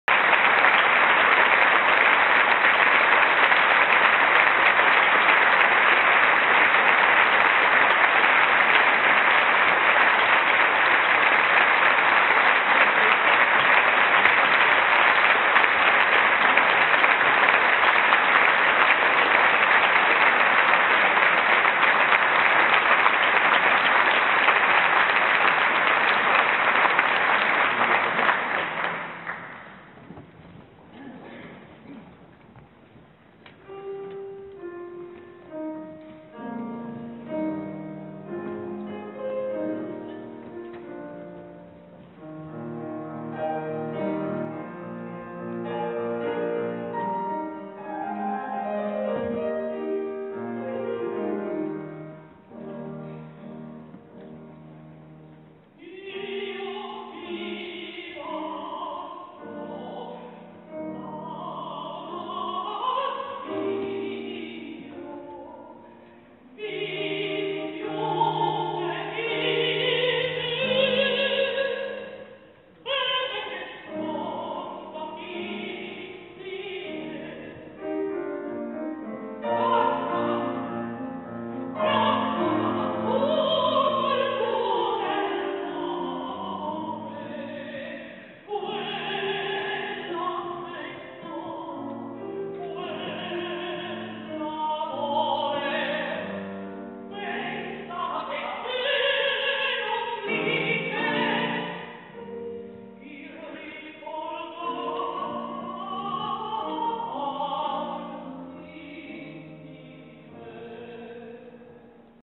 Fiorenza Cossotto [MM57-07]
Concerto di Fiorenza Cossotto al conservatorio di Torino